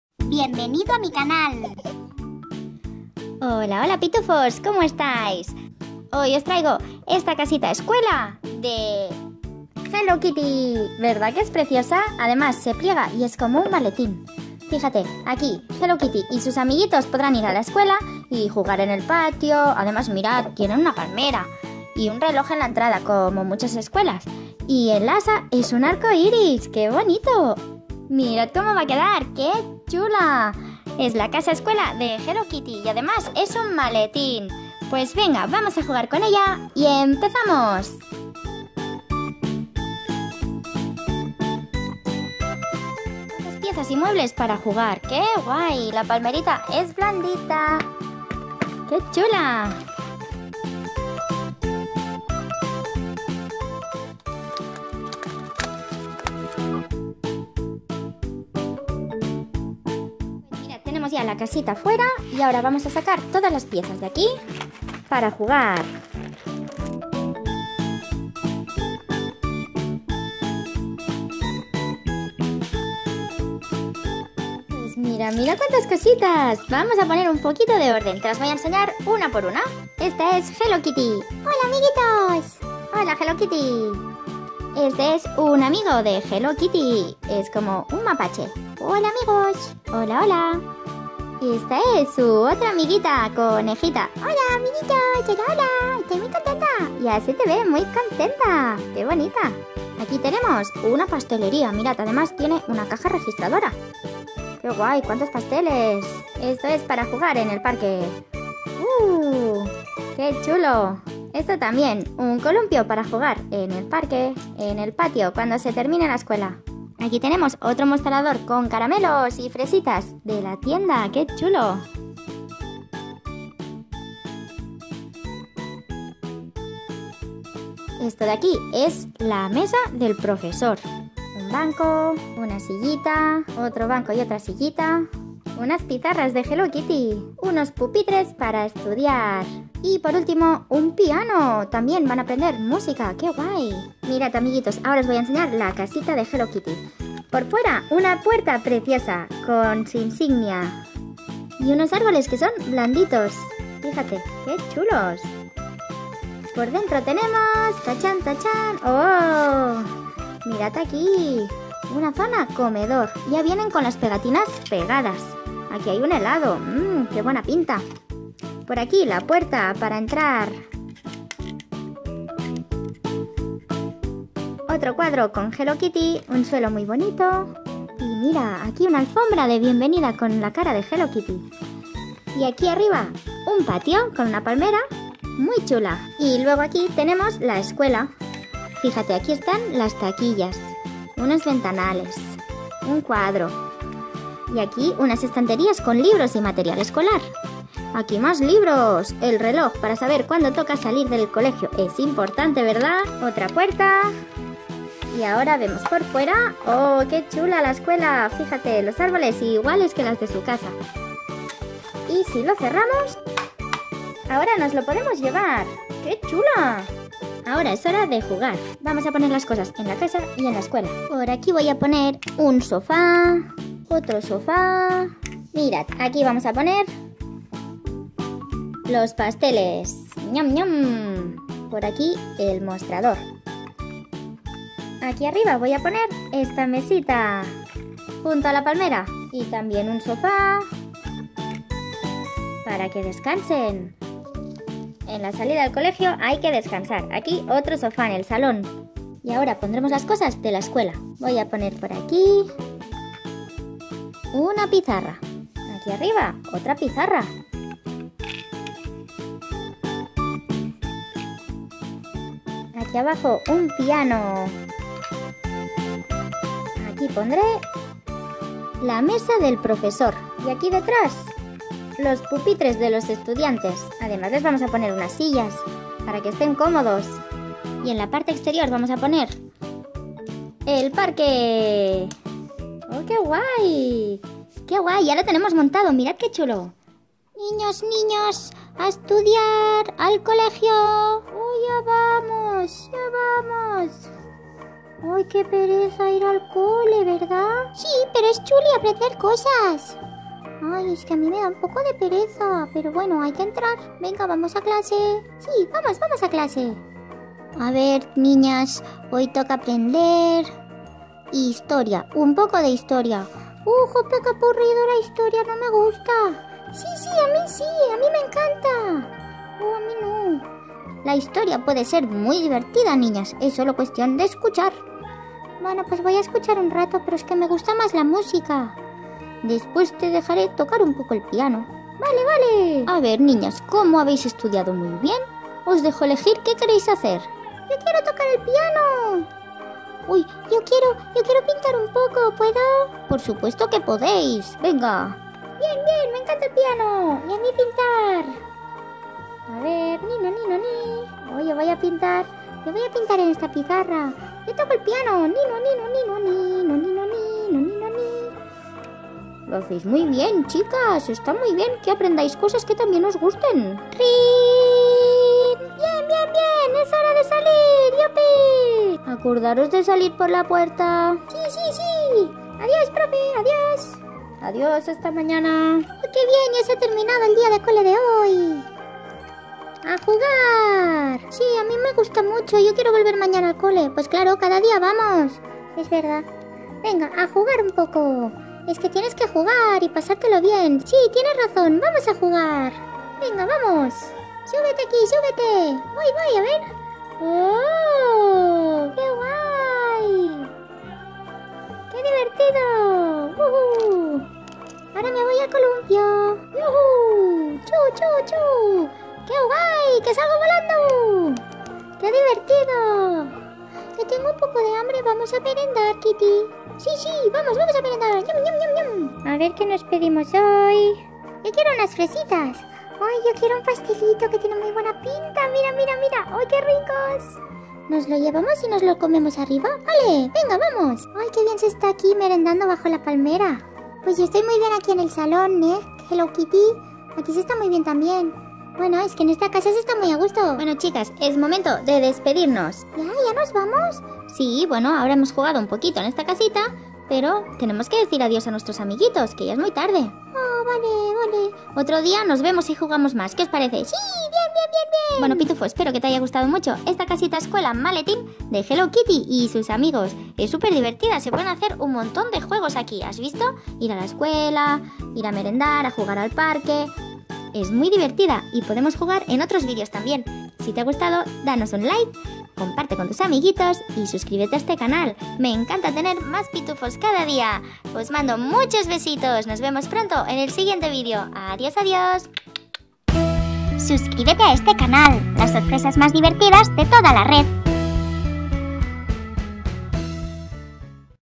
Jugamos con el maletín Casa-Escuela de Hello Kitty
Hoy os enseño este maletín casa-escuela de Hello Kitty y sus amigos. La montaremos y jugaremos con ella, ¡será súper divertido!

Este es un juego de casas de muñecas con juguetes de hello kitty, ella y su amiga irán a la escuela, aprenderán muchas cosas y luego irán al parque a jugar y a su casa a merendar.

¿Te gusta?

Un besito pitufooos!

¡GRACIAS POR VISITARNOS Y REÍRTE CON NOSOTROS!